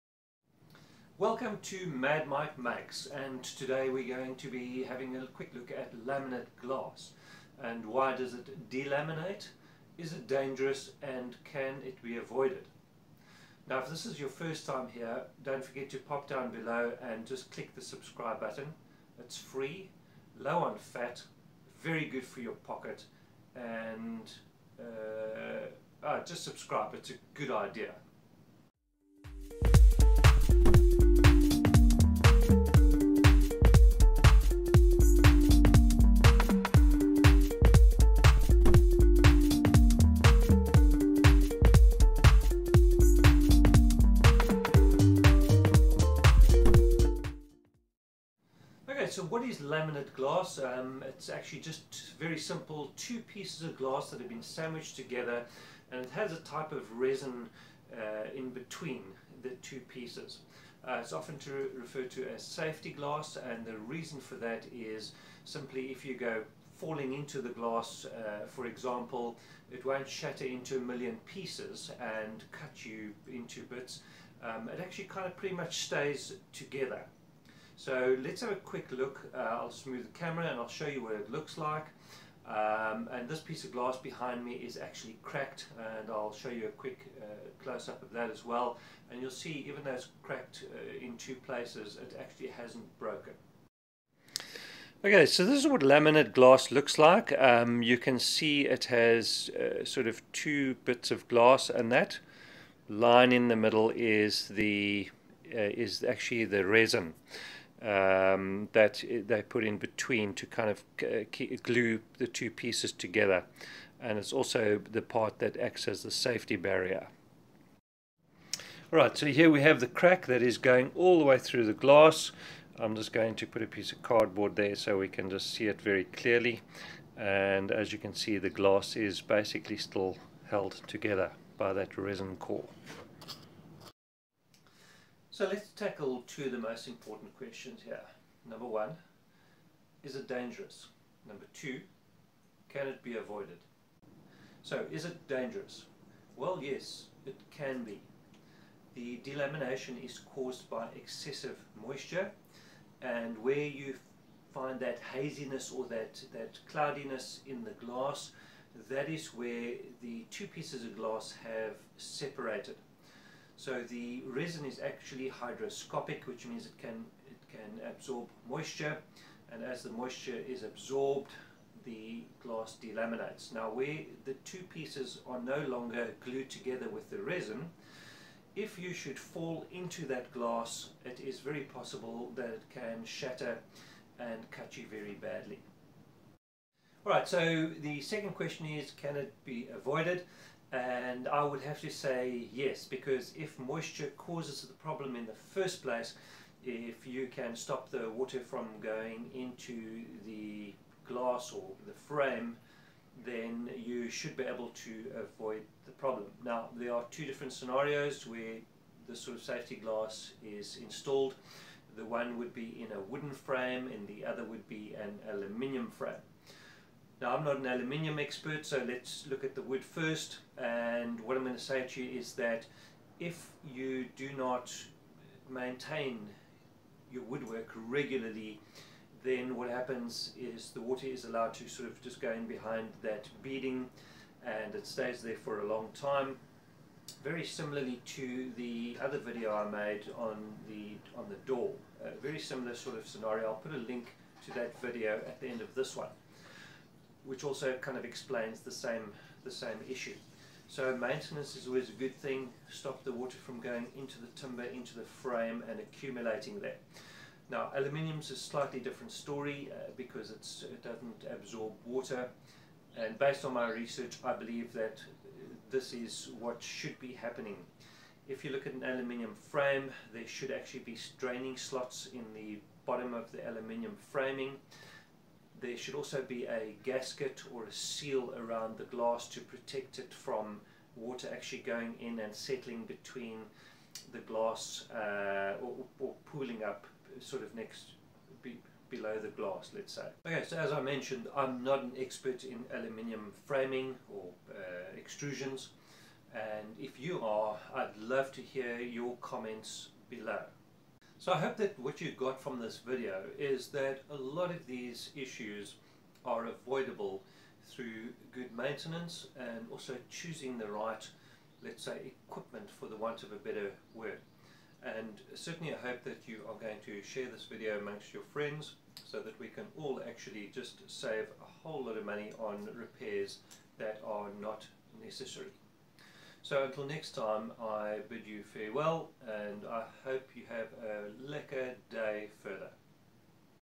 Laminated glass problem - How to avoid it
This is a fairly common problem that I've come across. It is only ever found in laminated glass. It normally happens where glass is installed in a frame like a door, window, or balustrade casing.

It is normally due to moisture becoming trapped and being absorbed by the resin causing delamination to occur between the resin and the glass.

The lack of maintenance is often the cause.